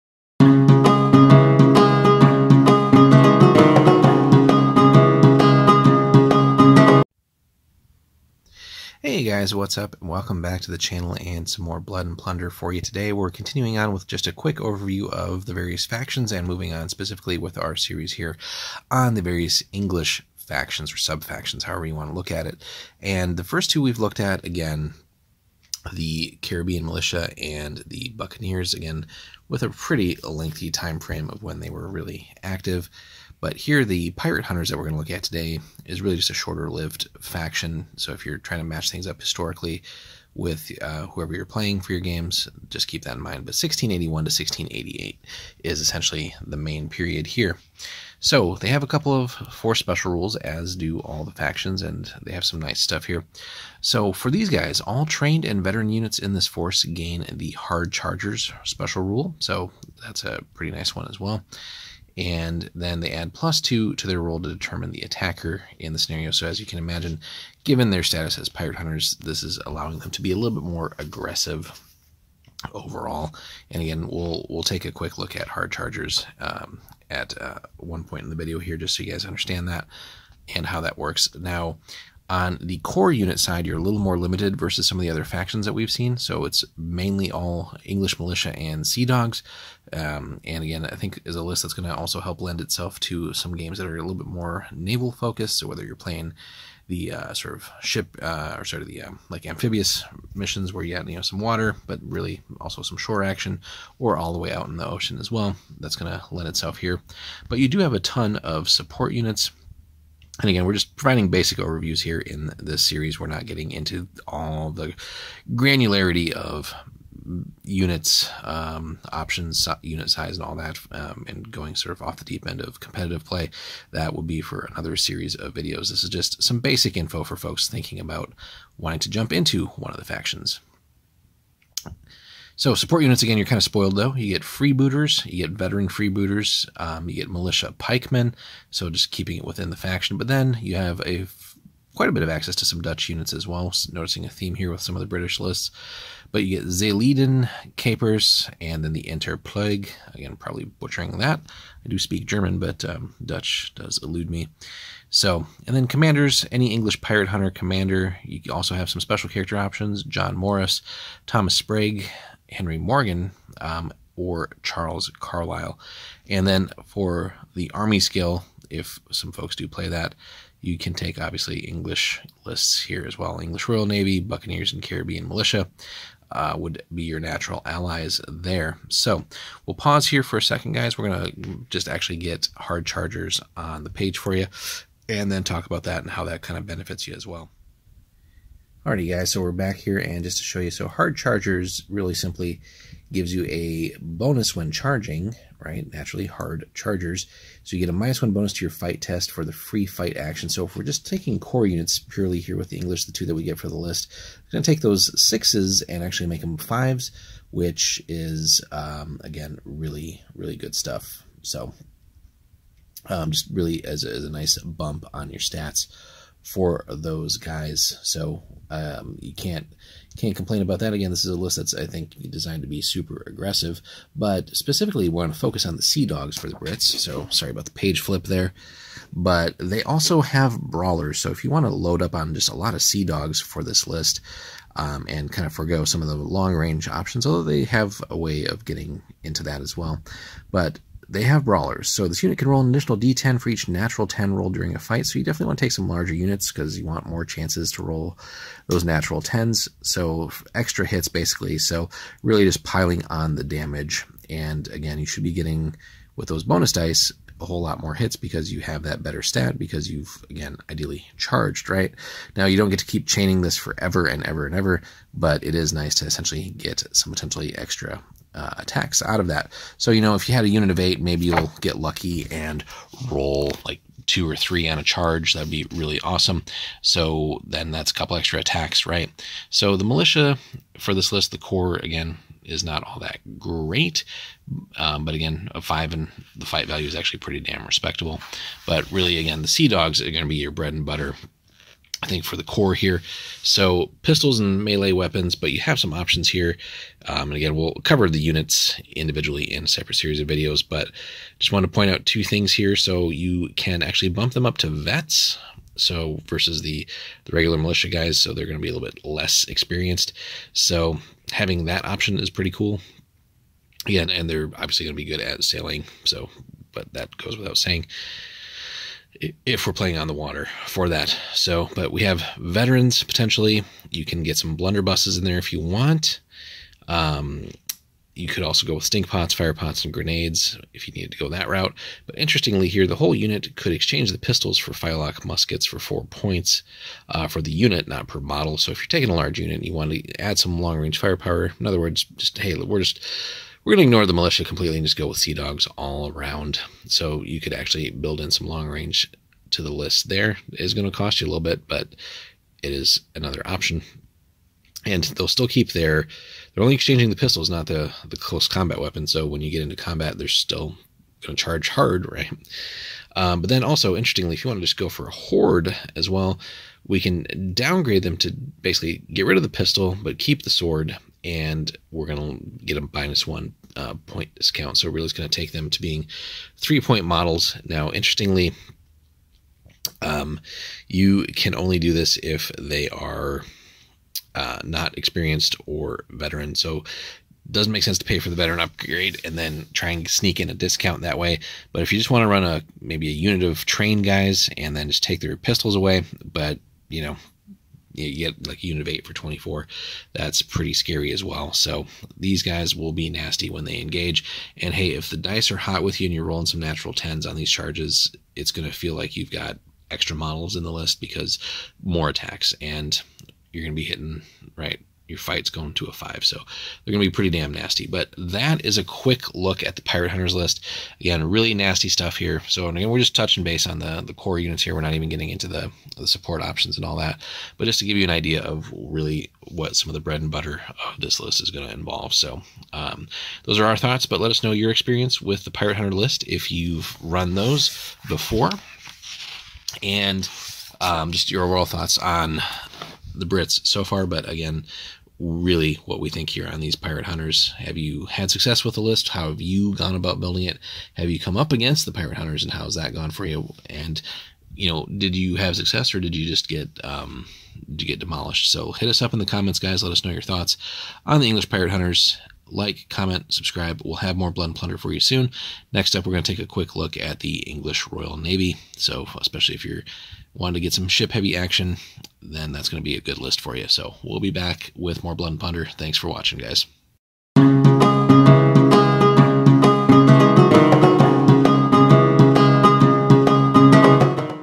Blood & Plunder - English Pirate Hunters
We take a look at the English Pirate Hunters subfaction!

A Robust Crew